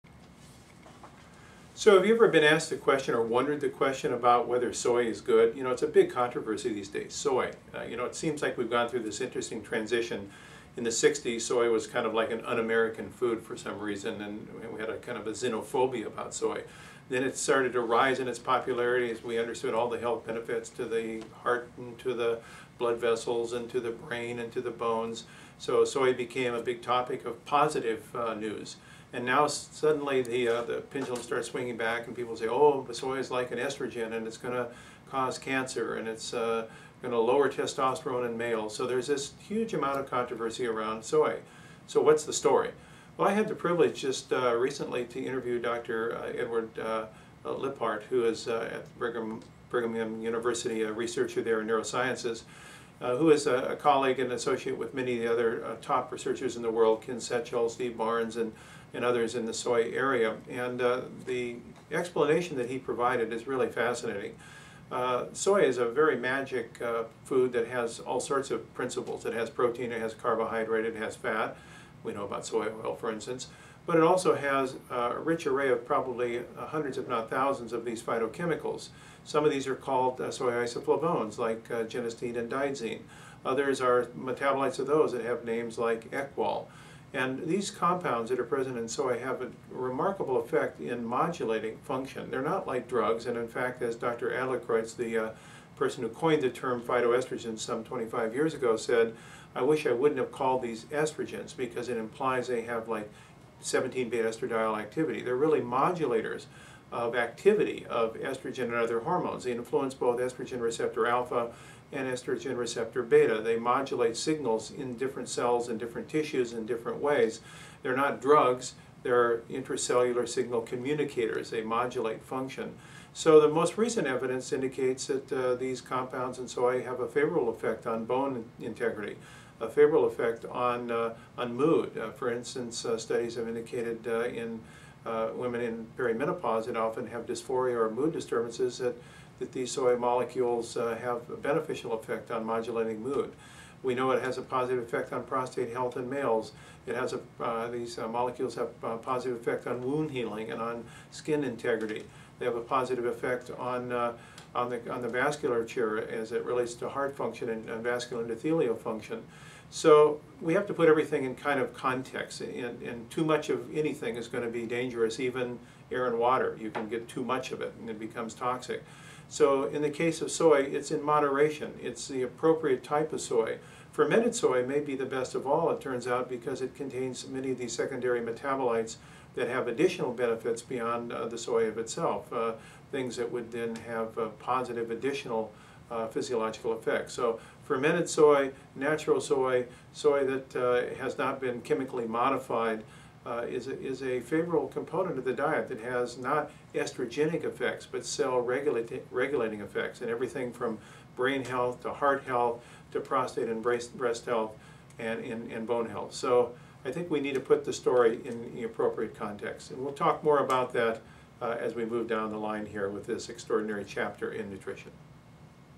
New research on soy
Dr. Bland discusses his upcoming interview with Dr. Edwin Lephart of Brigham Young University on new research on soy. Look for the interview in the July issue of FMU